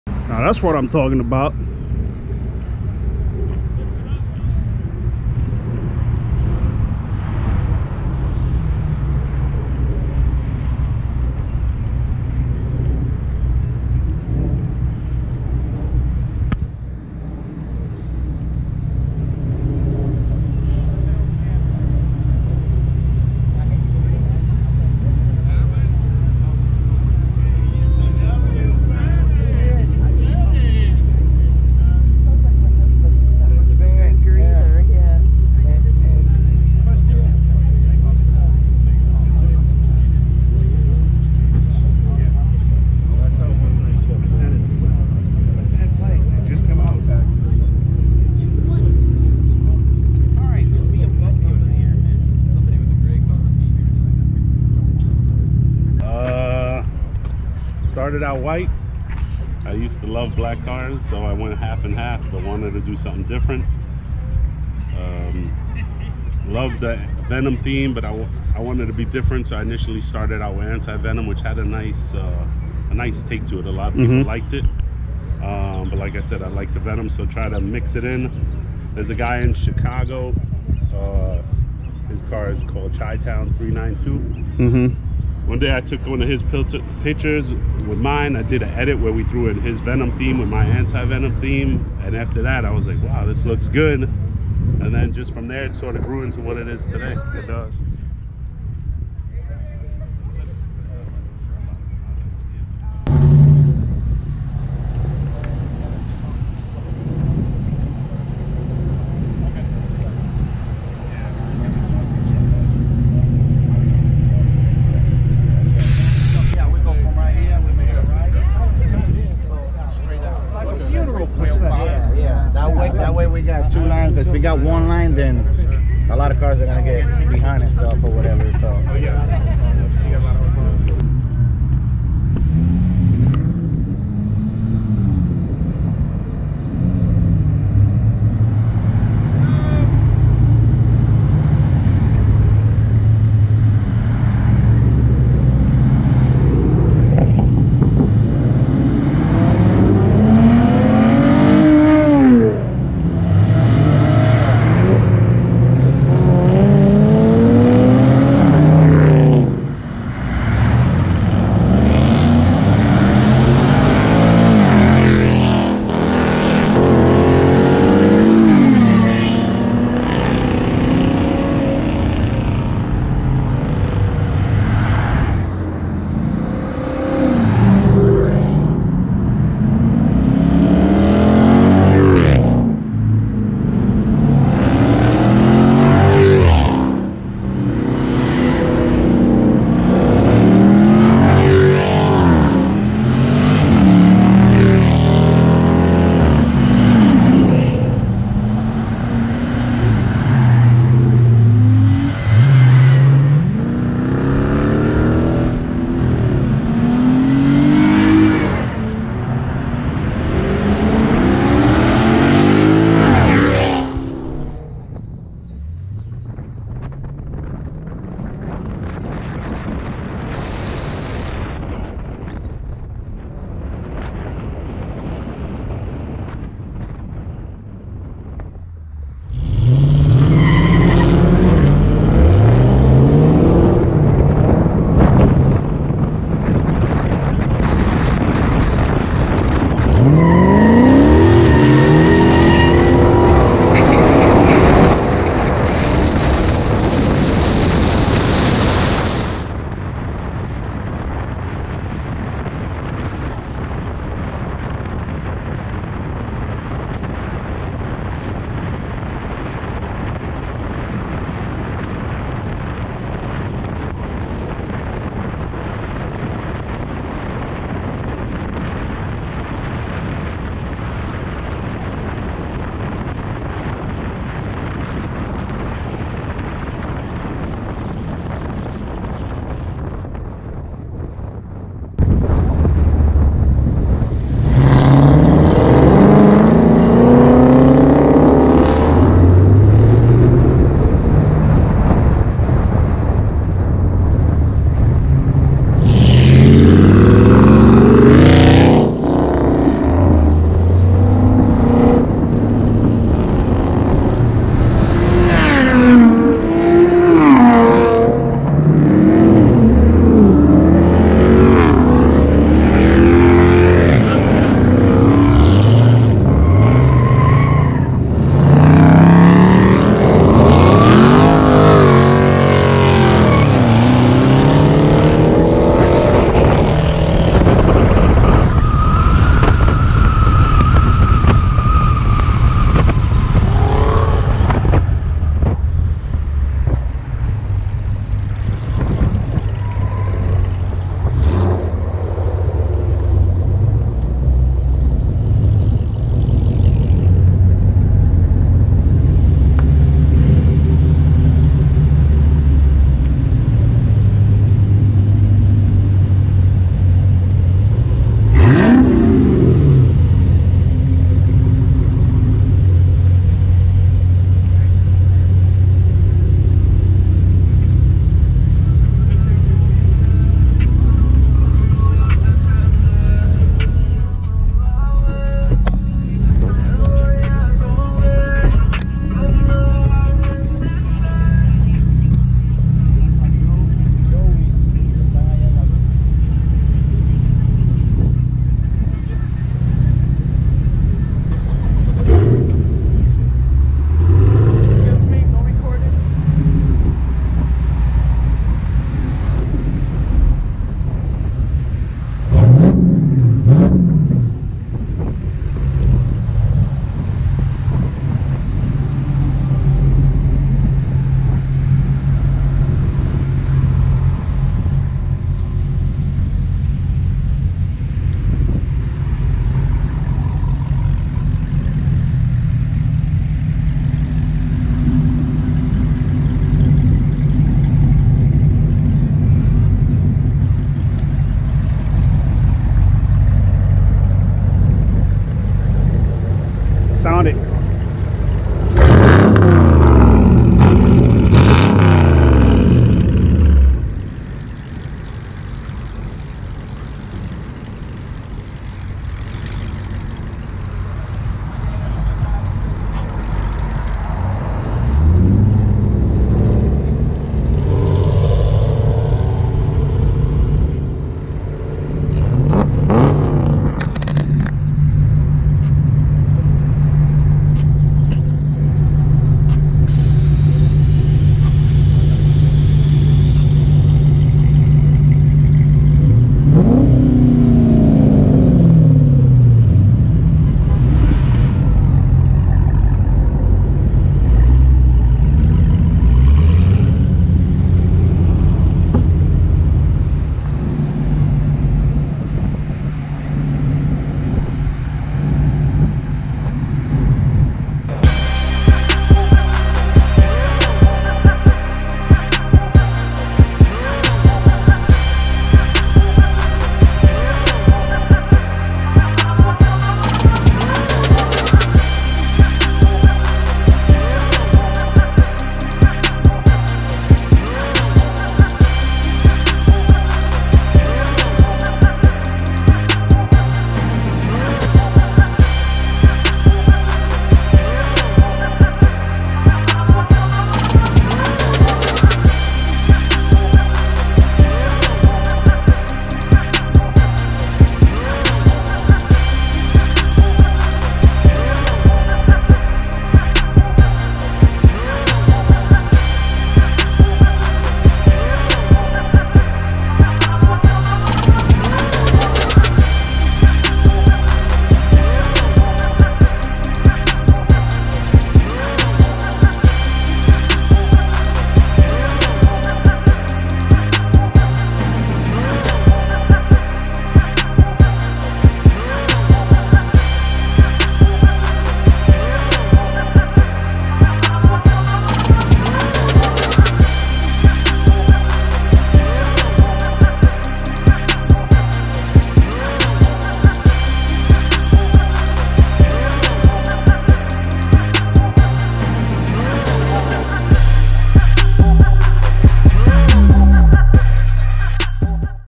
The Most Beautiful Dodge Charger Car Wrap | Meet and Cruise
Venom/Anti-Venom wrap on a Dodge Charger. One of the most beautiful wraps I've seen.